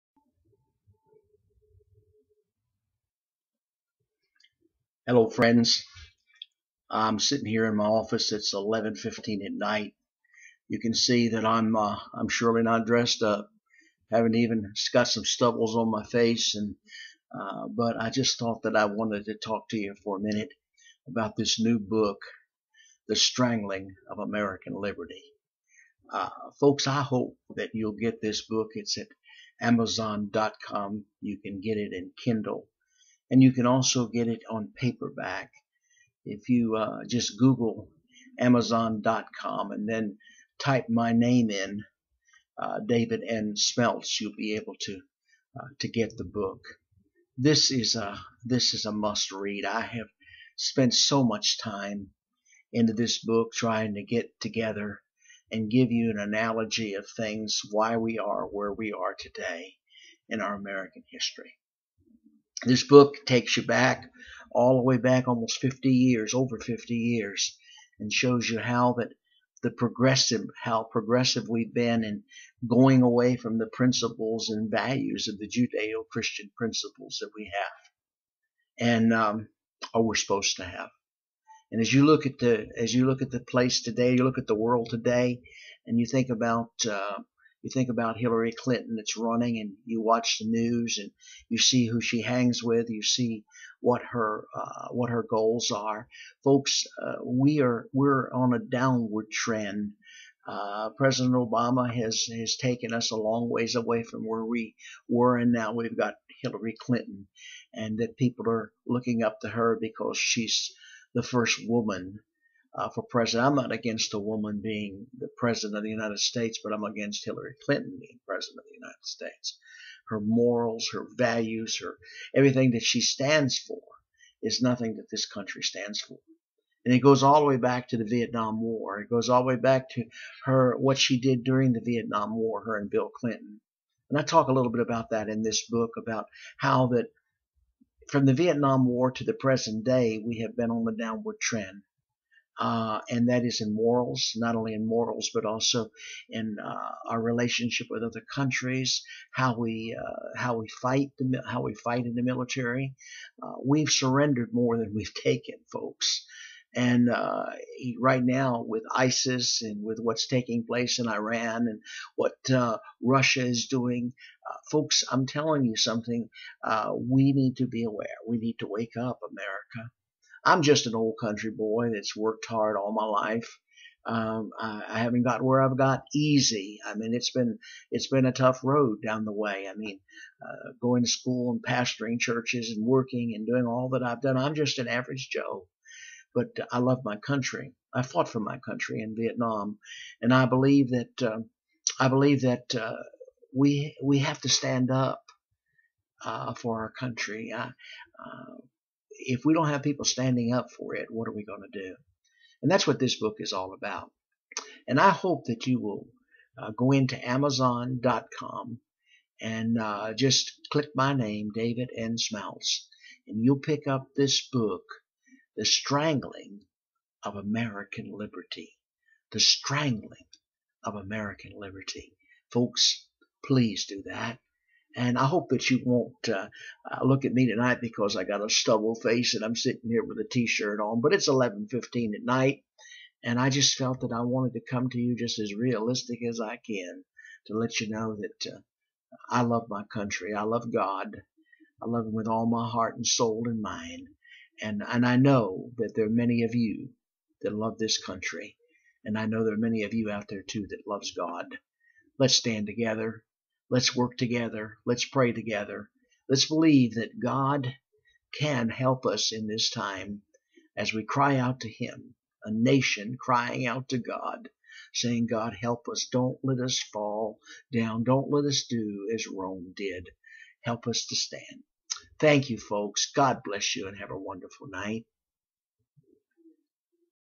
Strangling of American Liberty- David N Smeltz Sr
This is a promo on my new book (The Strangling of American Liberty) This video was done late at night when I just was burdened to talk with you all about this book. I was not dressed up.. I was just me!!!